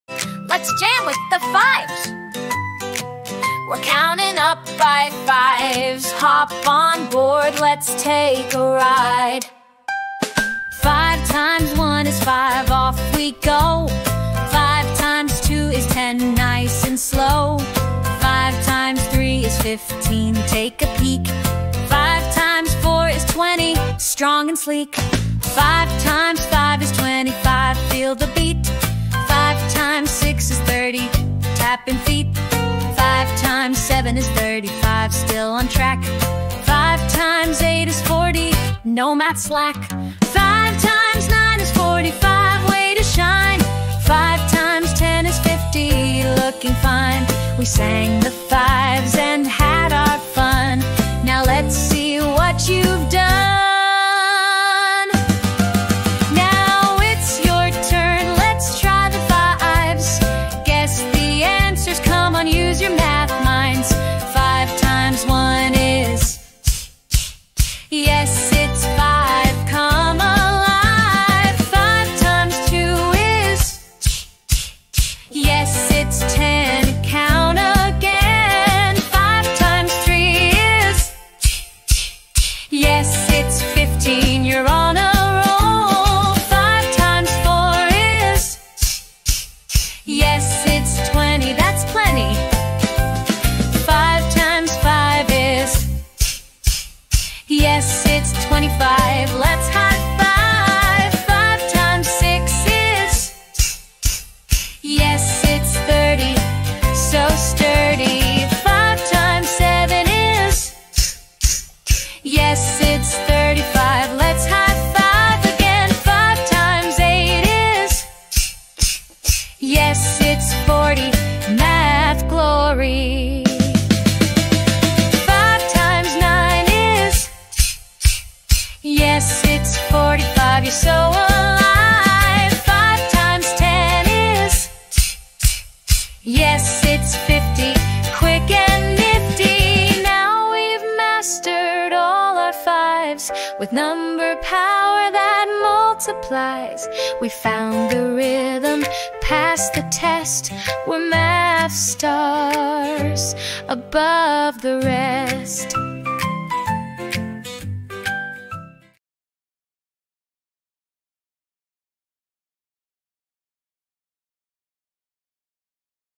Learn the 5 Times Table! 🎵 | Can YOU Keep Up? (Answer Challenge)
🎵 Let’s sing the 5 times table together — then it’s YOUR turn to answer!
This upbeat, interactive song teaches kids how to multiply by 5 with catchy lyrics, rhythm, and visual cues. After singing together, kids get a chance to test their memory with the built-in Answer Challenge — perfect for fun learning at home or in the classroom!

✅ Helps children:

Memorize the 5x table through music

Build confidence with response time

Engage with math in a fun, stress-free way!

👩‍🏫 Perfect for:

Teachers using music in math class

Homeschool parents

Kids learning multiplication through repetition